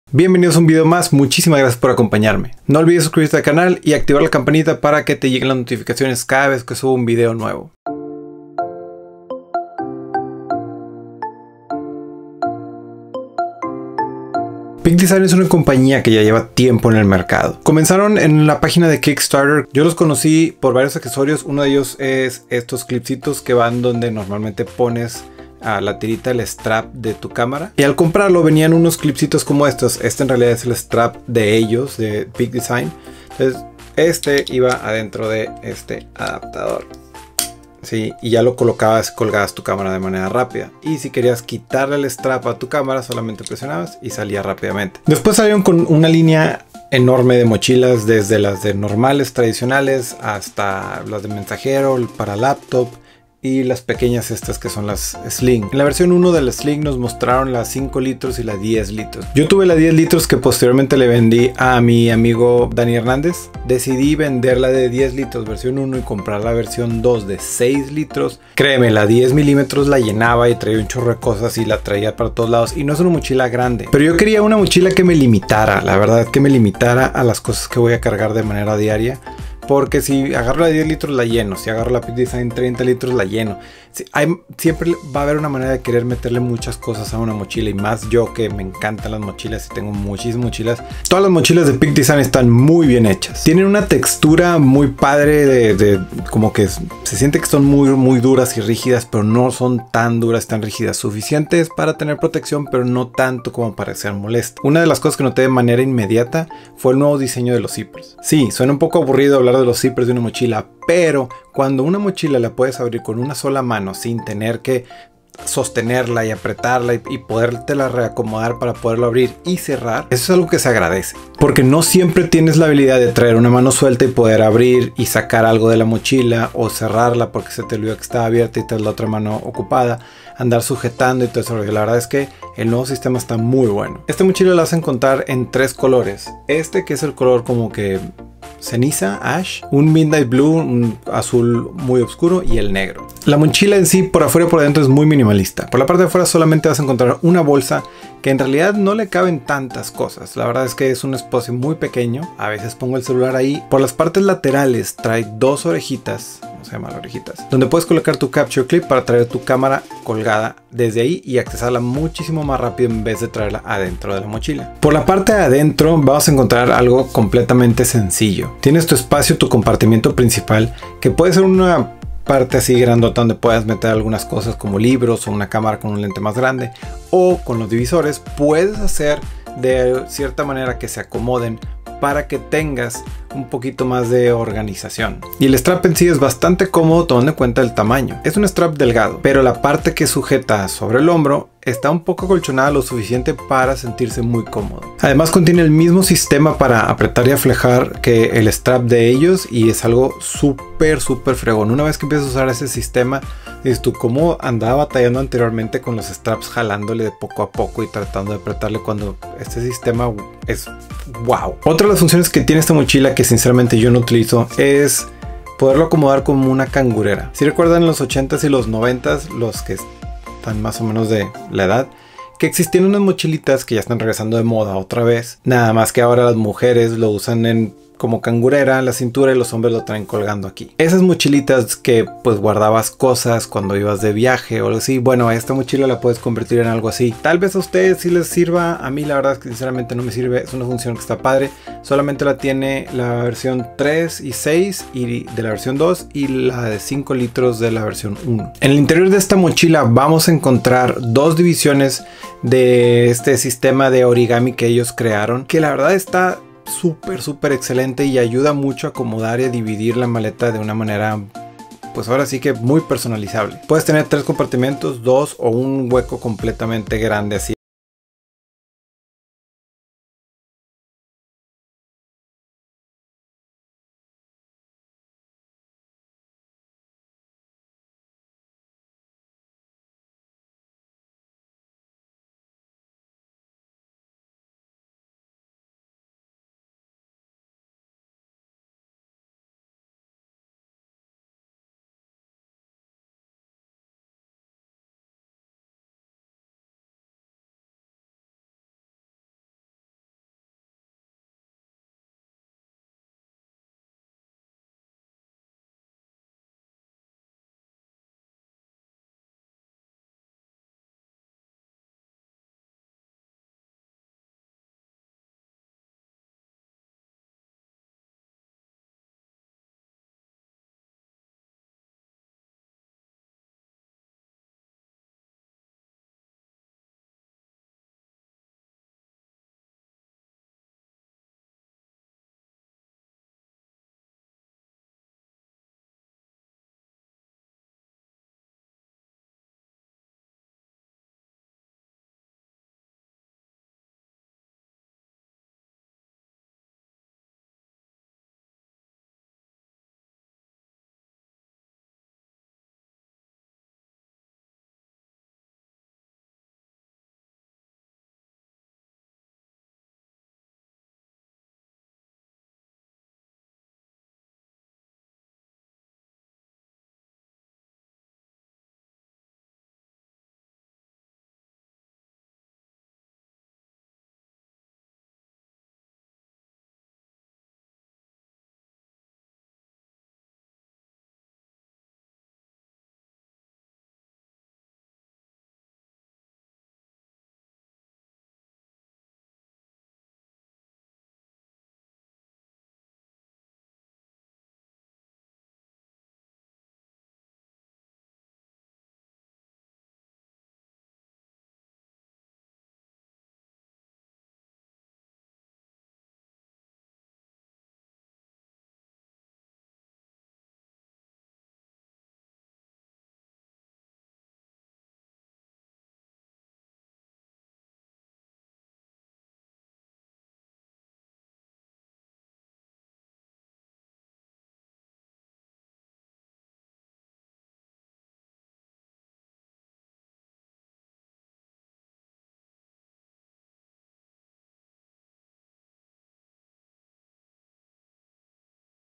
RESEÑA Peak Design 6L Sling
Una mochila para uso diario, minimalista y super cómoda, la Peak Design 6L Sling es una mochila cruzada, compacta para cargar lo esencial, y un poco mas...
Excelente para uso diario con artículos personales, así como con artículos de fotografía.

Artículos que eh colocado dentro :

1. Fotógrafo
Cámara Sony a7iii con lente Samyang 18mm 2.8
Lente Sony 85mm 1.8
Baterias
iPad Pro 11"
Cable y cargador de iPad Pro
Airpods Pro
Tarjetas de Memoria

2. Creador de Contenido
iPad Pro 11"
Teclado Logitech Inalámbrico
Stand para iPad Pro de Amazon Basics
Estuche con cables y adaptadores para iPad Pro

3. Papá
Pañales
Toallitas humedas
Vasos de jugo para las niñas